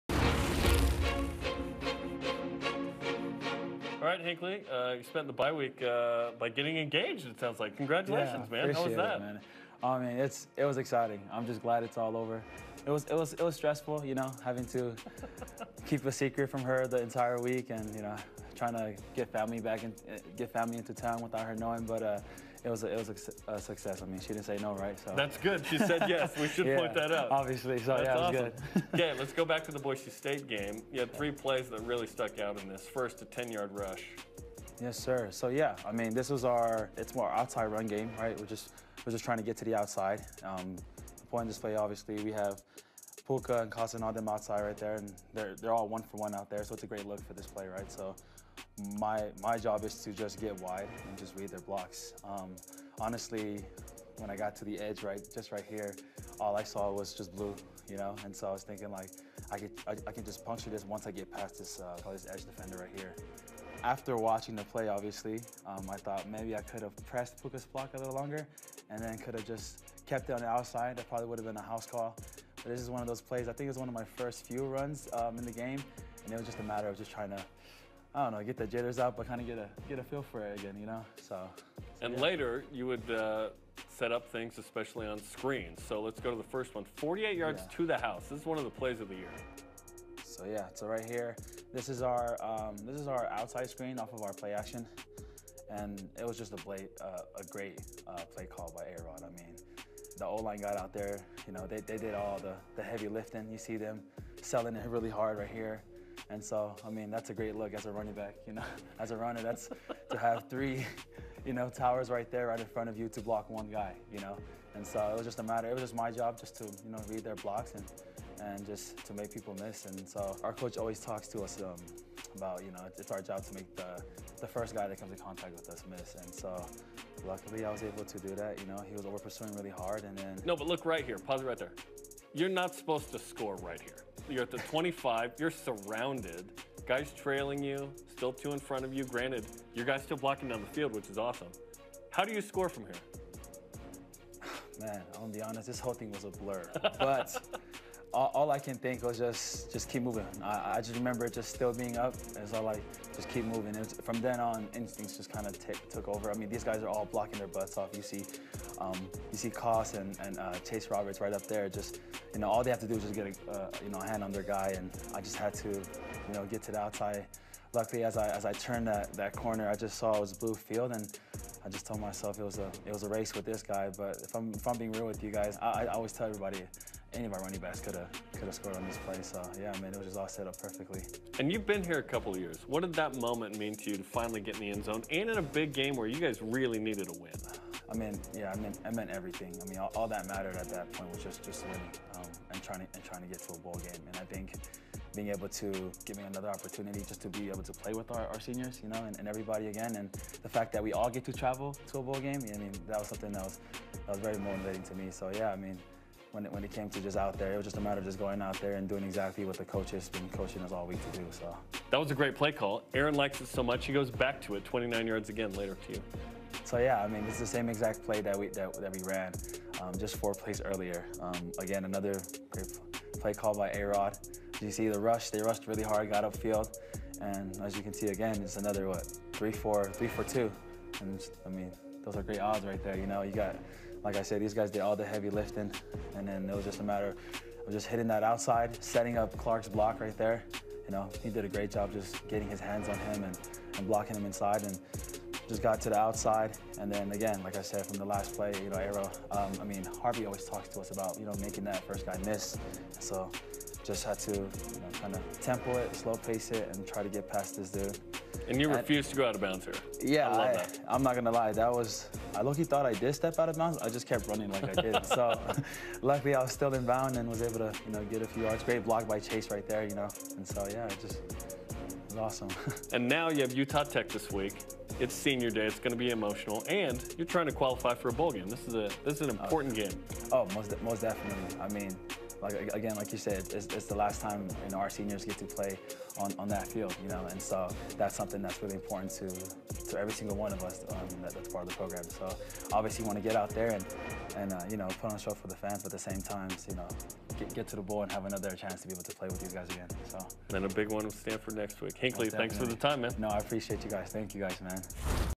The Film Room with Hinckly Ropati
After a breakout performance against Boise State, Hinckley Ropati joins Jarom Jordan to break down some of his biggest plays against the Broncos.